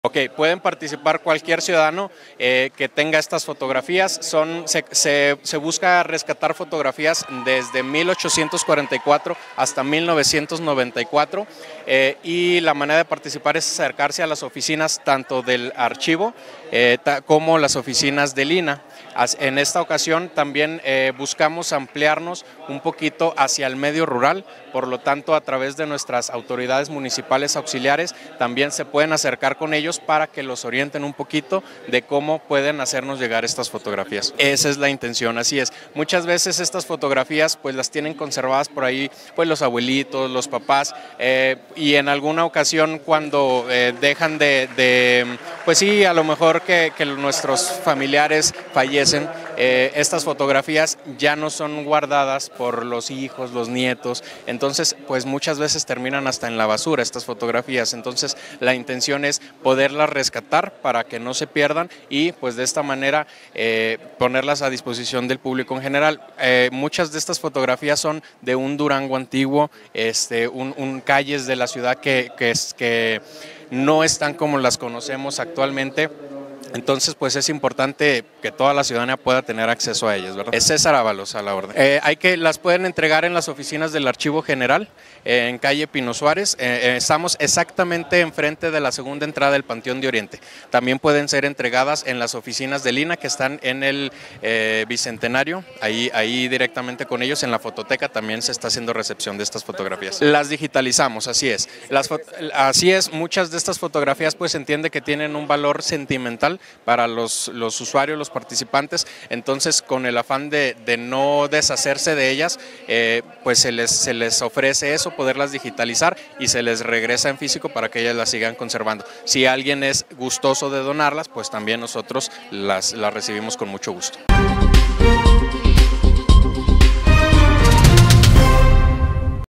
Se lanza la convocatoria por parte del archivo histórico municipal
Se lanza la convocatoria por parte del archivo histórico municipal para que los ciudadanos puedan llevar sus fotografías antiguas que daten desde 1884 hasta 1994
César Ávalos
Director del Archivo Histórico Municipal